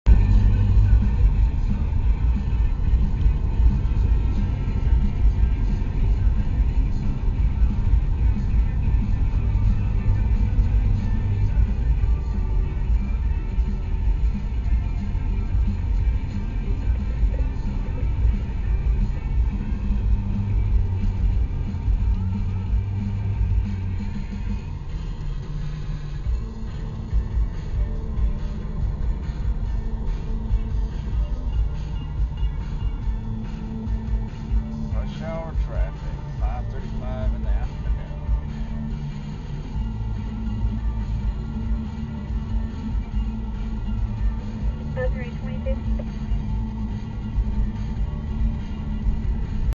Rush Hour Traffic
Driving through the beginning of evening rush hour traffic one day in Houston